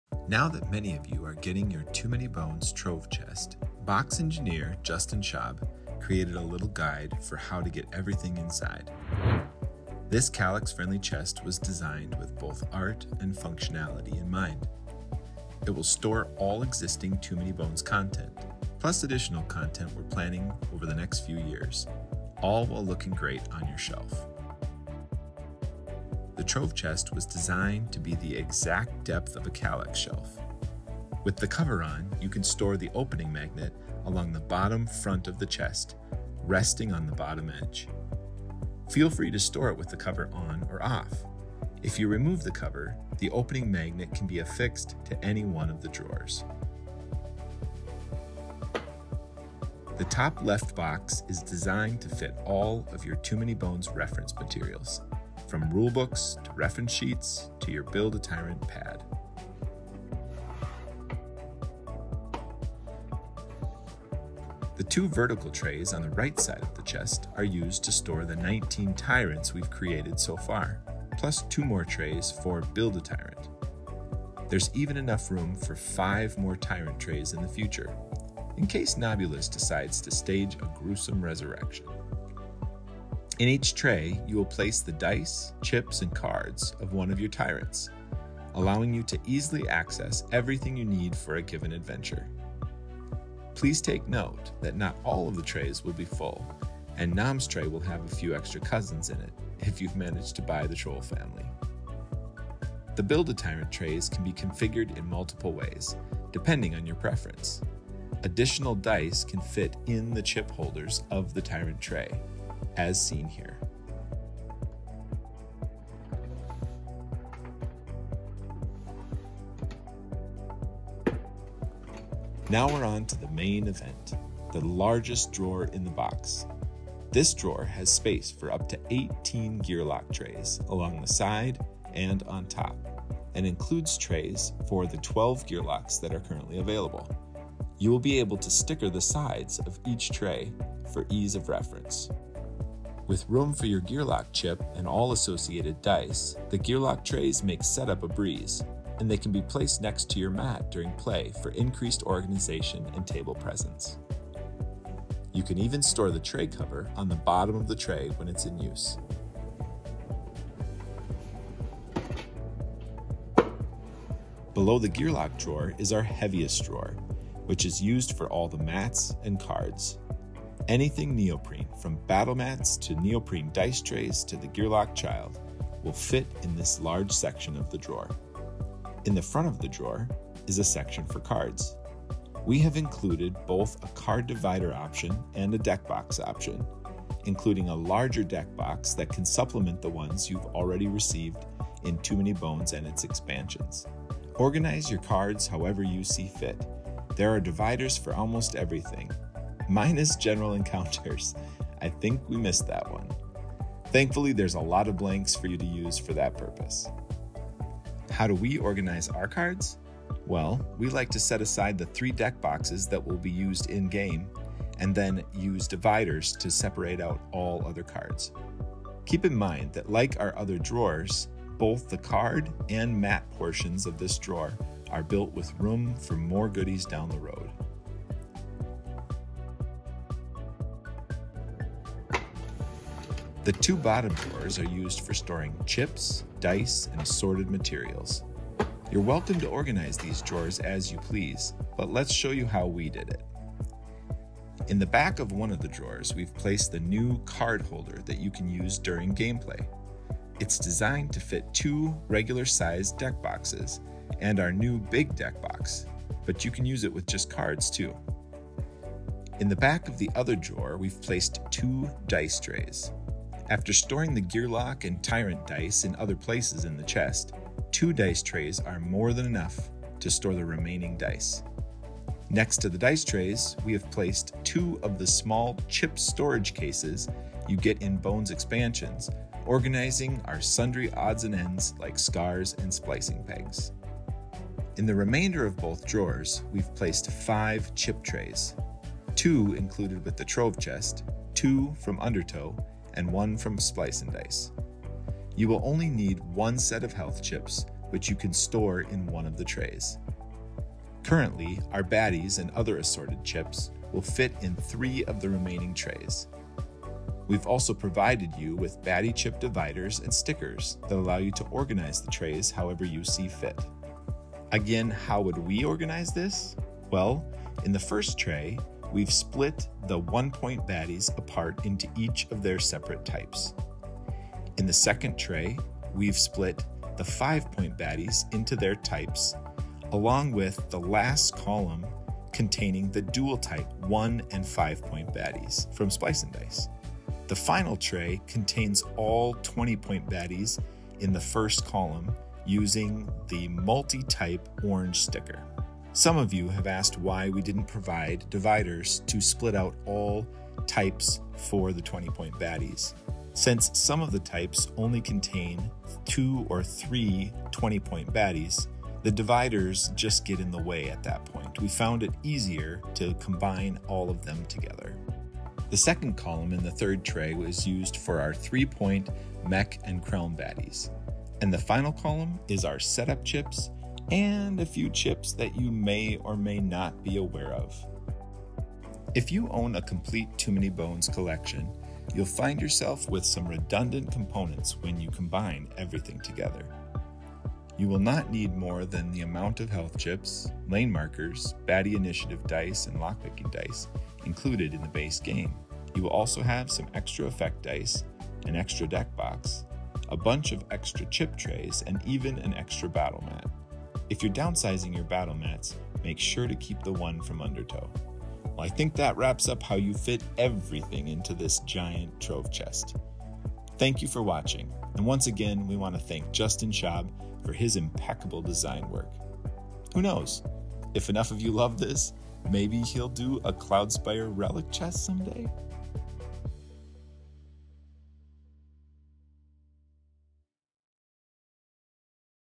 Too Many Bones Trove Chest Version 1.0: Storing Your Collection
How does one store their Too Many Bones collection in the Trove Chest? Let us show you in this complete video breakdown. Note that the Trove Chest is meant to be a fully customizable storage solution for TMB - but this is Chip Theory's official recommendation on how to store your collection.